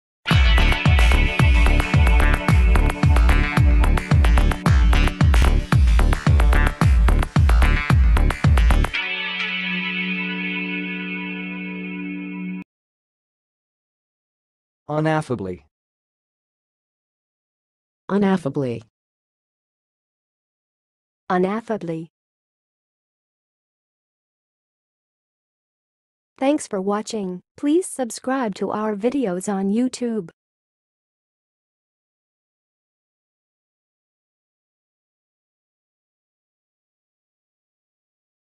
How to say "unaffably"! (High Quality Voices)
Watch in this video how to say and pronounce "unaffably"!